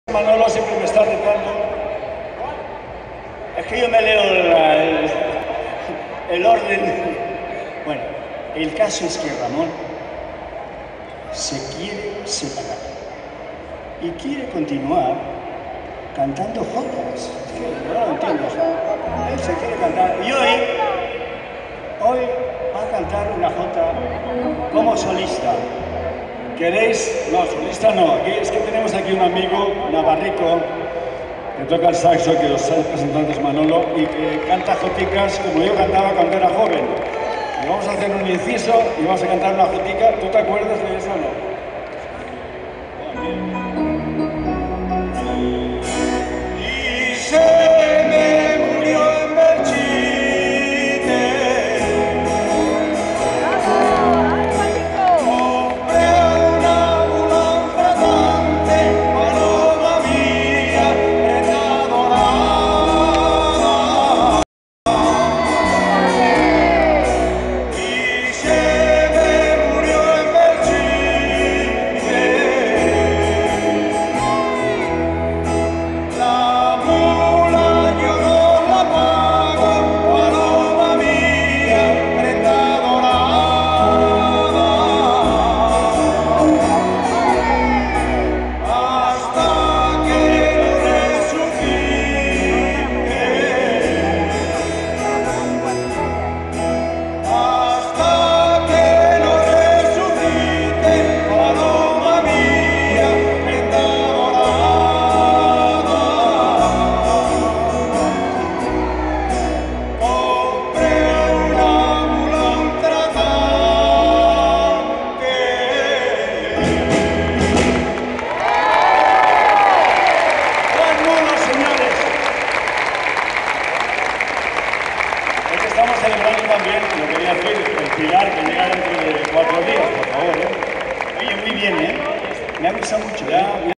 Dúo Dinámico (Vídeo 5)
Concierto del Dúo Dinámico en Boadilla del Monte el día 07/10/2018